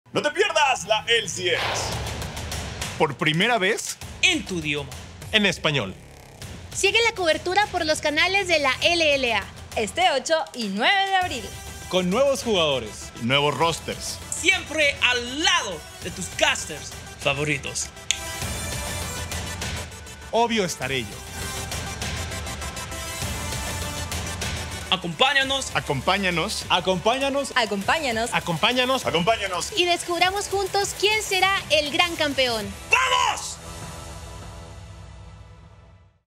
¡Tenemos una cita con la Final LCSEnEspañol! | Abril 8 y 9 | Esports | League of Legends
Todo el talento de nuestra región y Raafaa te invitan al cierre de temporada de LCS. Por primera vez en español y en nuestros canales LLA.